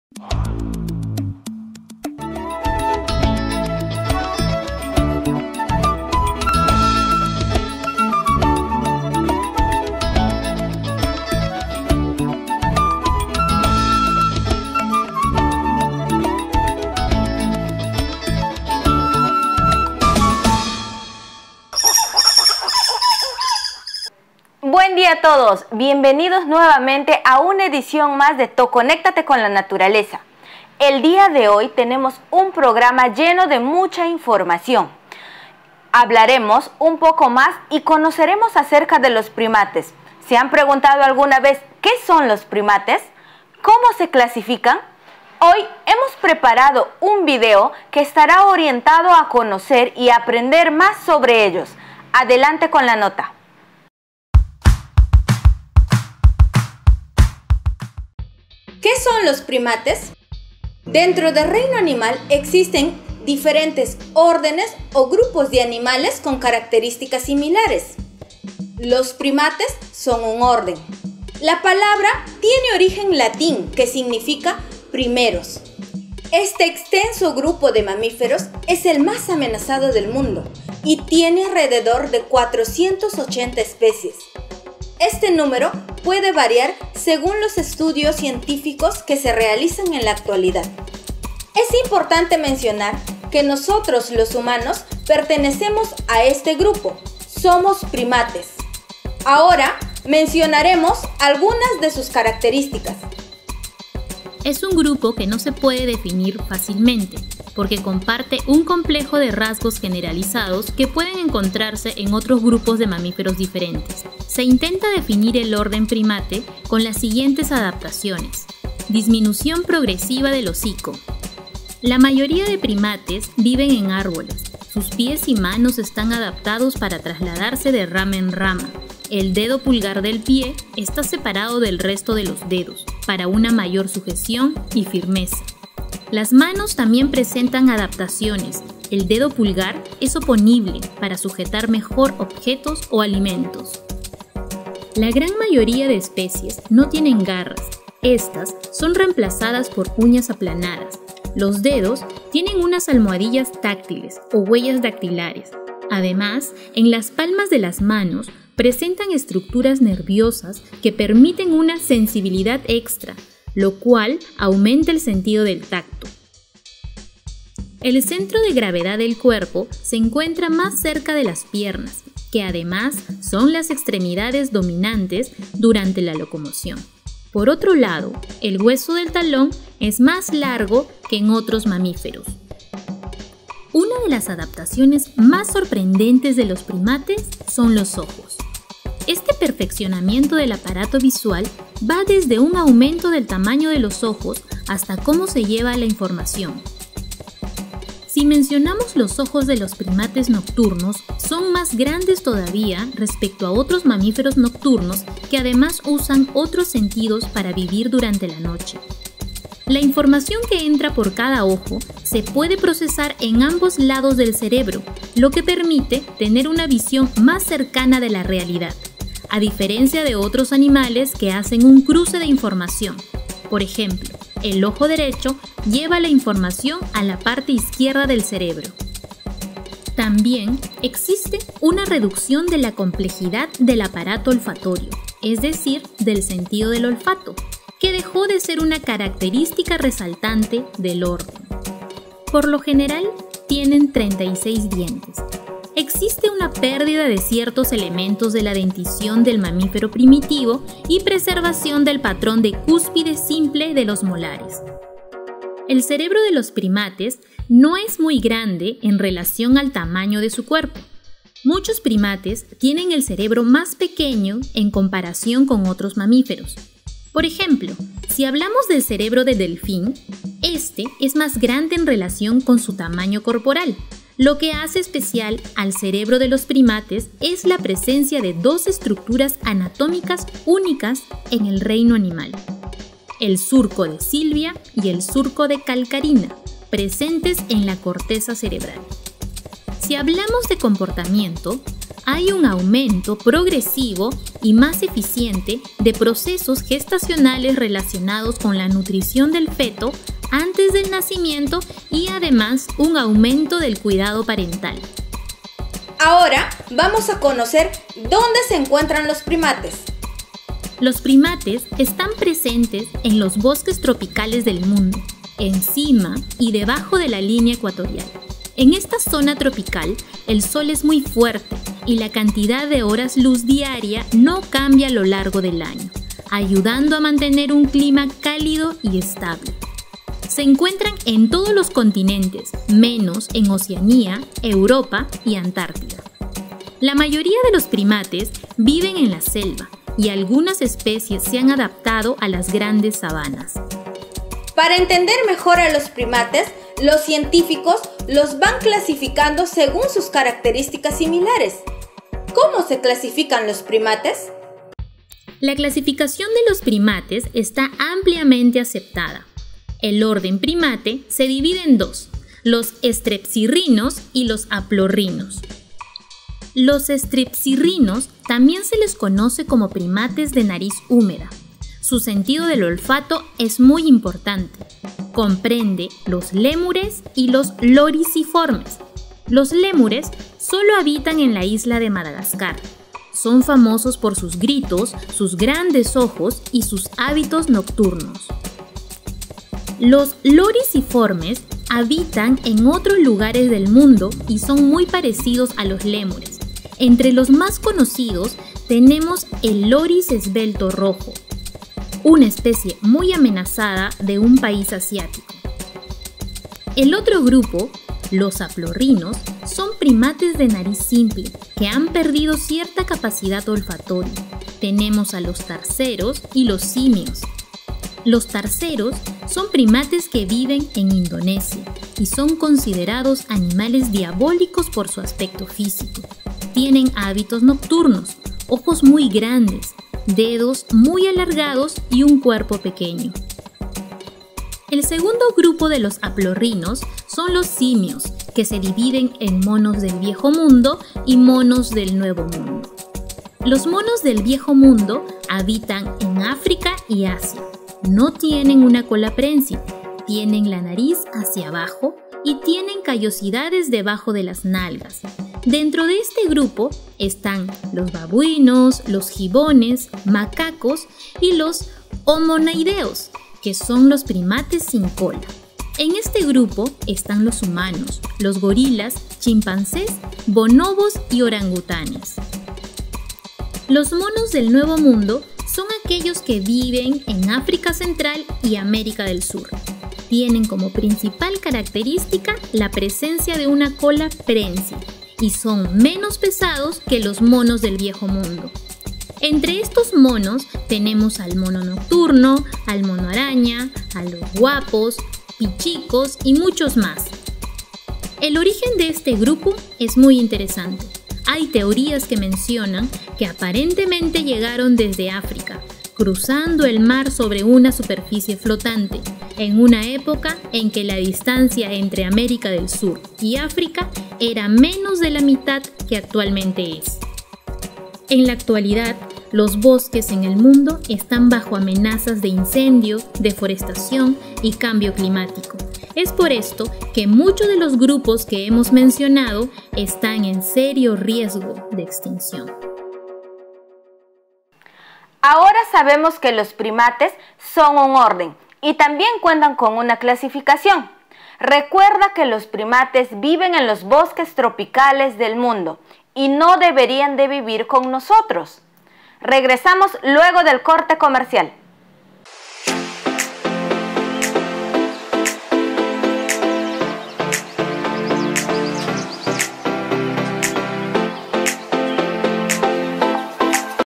DESCUBRE ¿Qué son los PRIMATES? I Los mamíferos más amenazados del mundo
Dentro del reino animal existen diferentes órdenes o grupos de animales con características similares. Los primates constituyen un orden muy importante, en esta edición de ValoraTuFauna aprenderás sobre ellos.